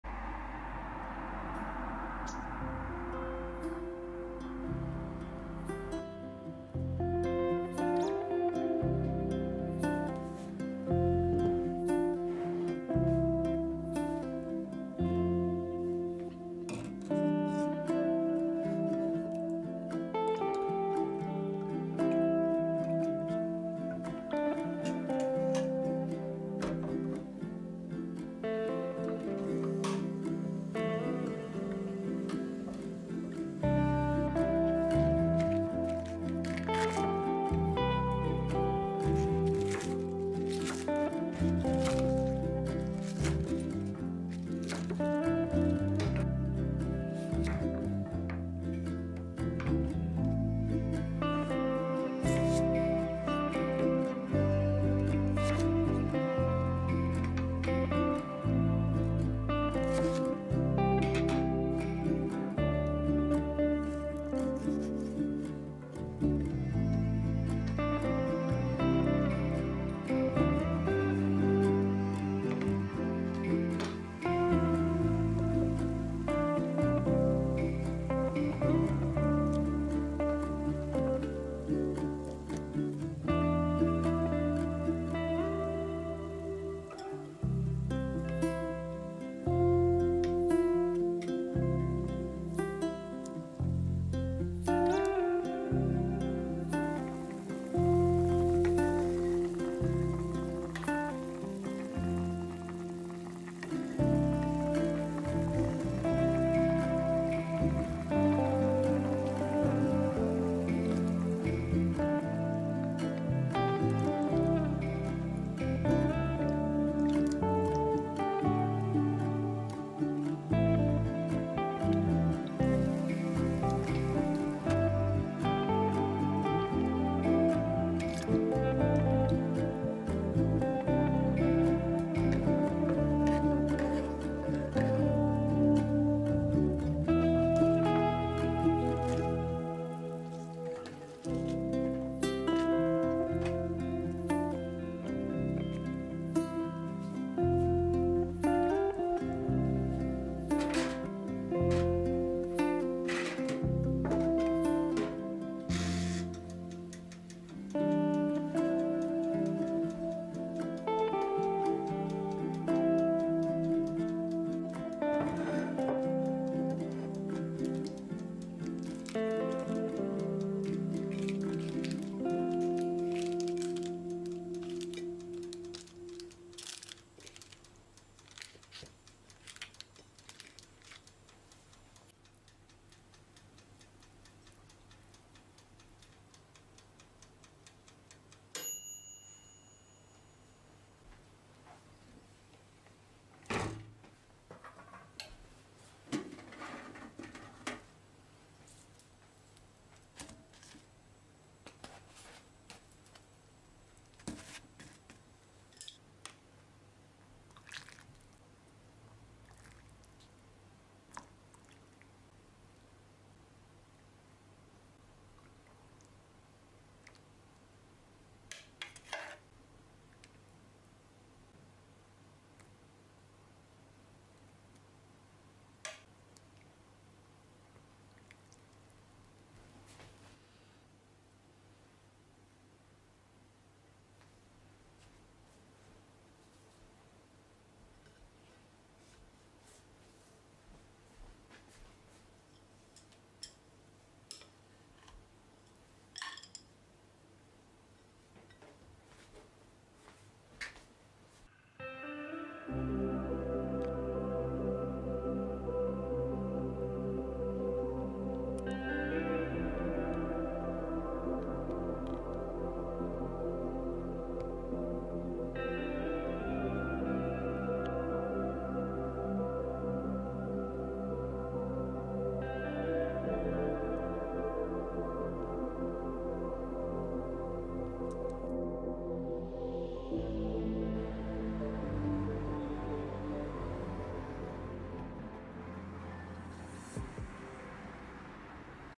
2021年元日、お雑煮と寿司で新年をスタートしてみた「１人暮らしクリエイター」
2021年 元日

１人静かな年越しを過ごしまして、お正月なのでお雑煮を作ってみました🍳🔪🎽

年末が死ぬほど忙しかったので、３連休はゆったりのんびり過ごそうと思います🎵

とは言え、YouTubeの撮影しなきゃだけど…😵

雑煮に使った材料

鶏肉
里芋
小松菜
大根
椎茸